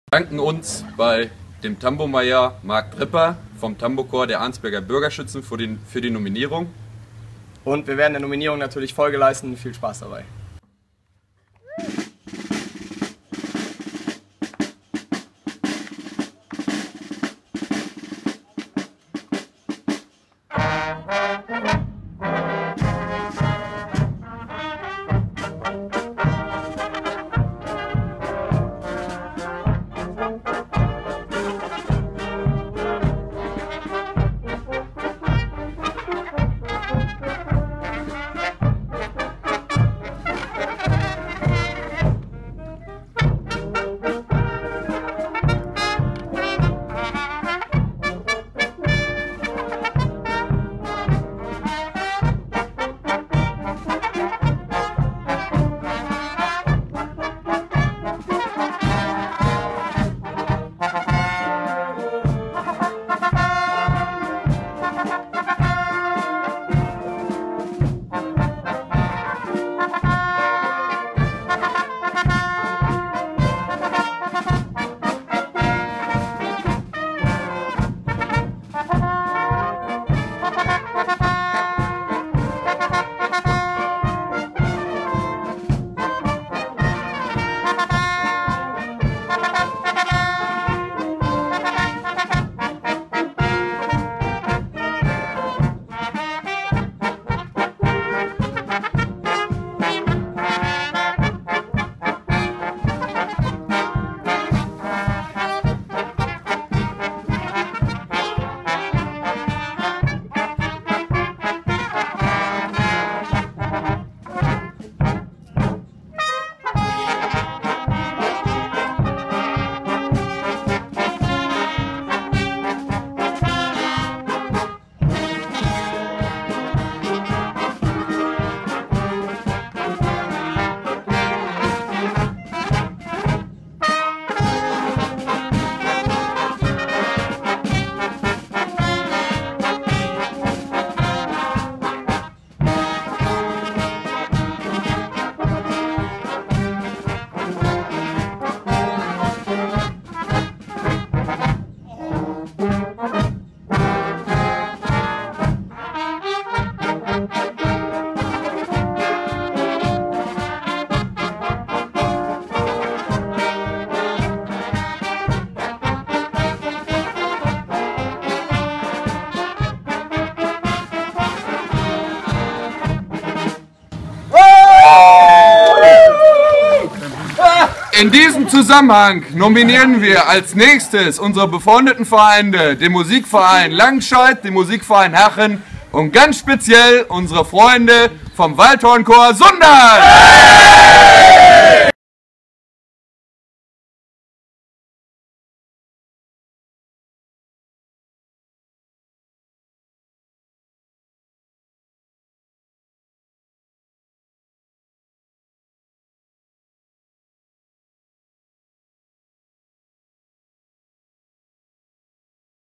Cold Water Challenge 2014 - MV Cäcilia Hövel
Unsere Antwort auf die Nominierung durch das Tambourcorps der Arnsberger Bürgerschützen.

Neu nominieren wir die Musikvereine aus Hachen und Langscheid sowie das Waldhorncorps Sundern